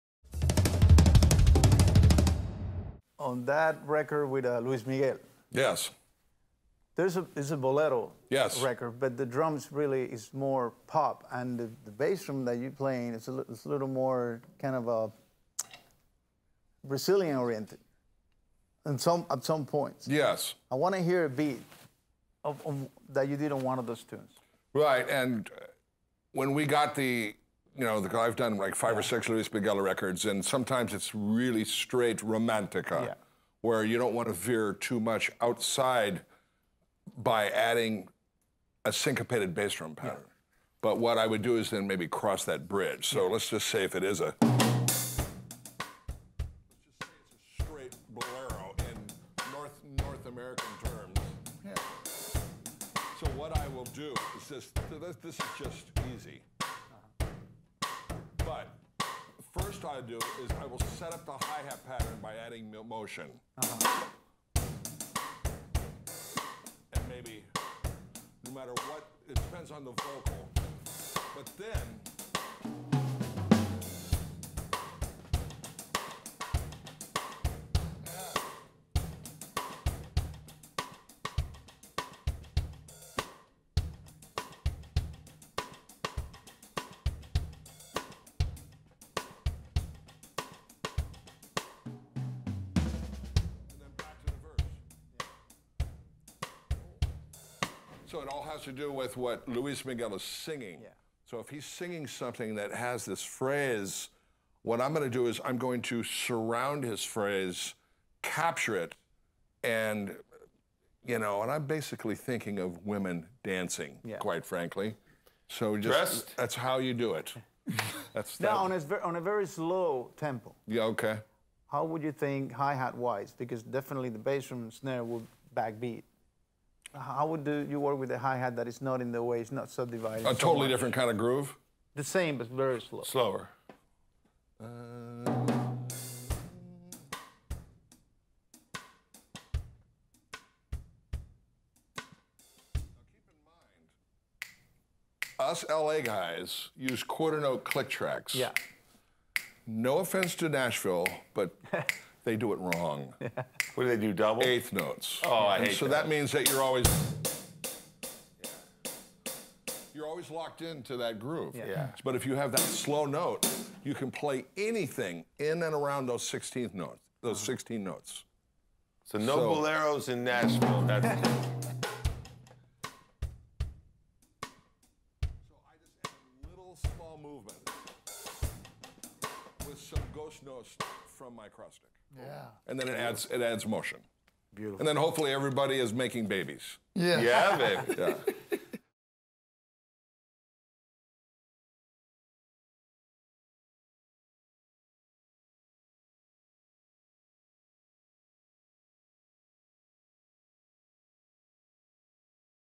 Jimmy Branly asks JR Robinson to demonstrate his signature drum pattern
As part of his "Outside The Box" tribute to legendary studio players, musician/producer David Garfield (Smokey Robinson, Cher, Steve Lukather) recently brought Gregg Bissonette, Jimmy Branly, JR Robinson and other top drummers to DrumChannel studios for an exclusive and unique recording session.

In a Q&A clip from this multi-part series, Latin drumming specialist Jimmy (Gonzalo Rubalcaba, Gloria Estefan, Celia Cruz) asks session-king JR (Michael Jackson, Barbra Streisand, Quincy Jones) to discuss and demonstrate his signature drum pattern from a recent Luis Miguel record. JR explains how the counterpoint, syncopation and ghost notes between the bass drum, hi-hat and cross-stick support the vocal track in this insightful, informative, inspiring and authentic "Secret From The Pro's".